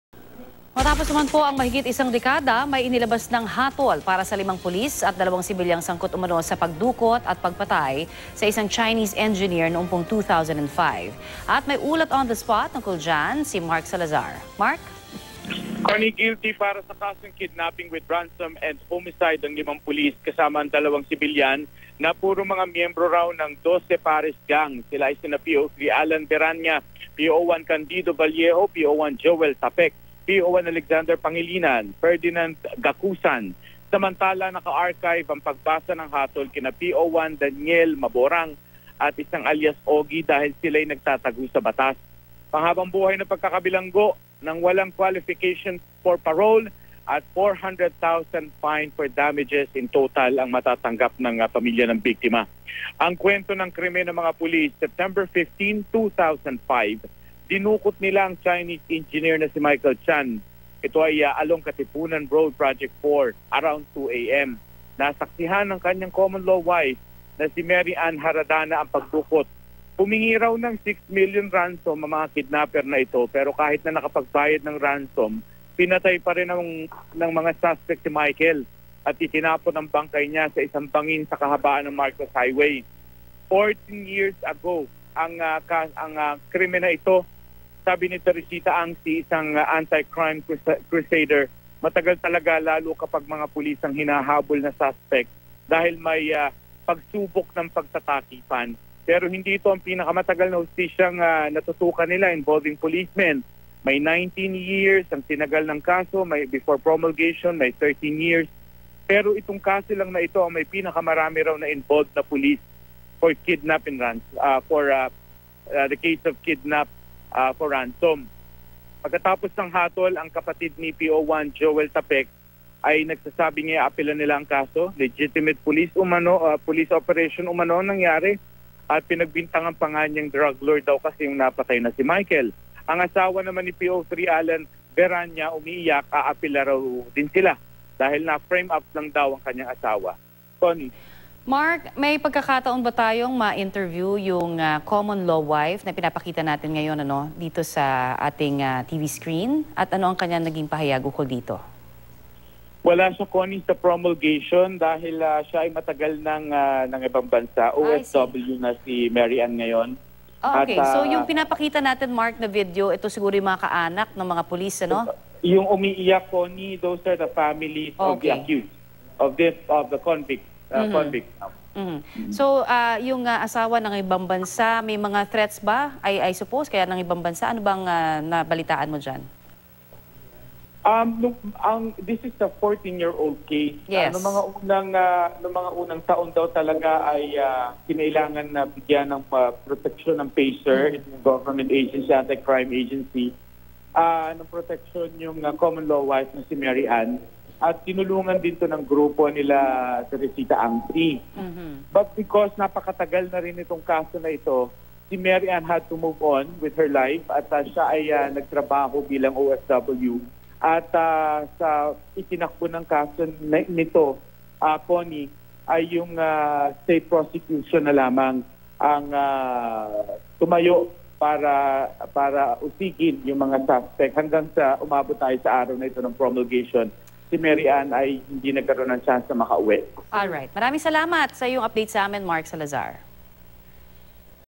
BT: Pitong miyembro ng isang gang kabilang ang limang pulis, hinatulang...
BT: Pitong miyembro ng isang gang kabilang ang limang pulis, hinatulang guilty sa kasong kidnapping with ransom at homicide